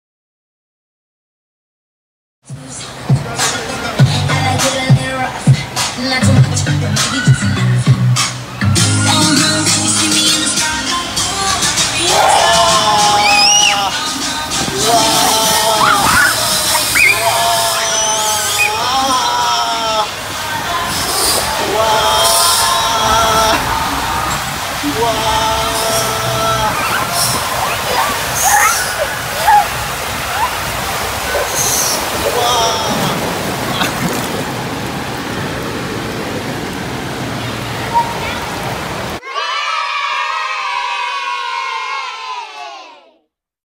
Toddler in Water-Park, Going down Big Water Slide , Fearless Marcelo Having fun in water park
Marcelo Toys & More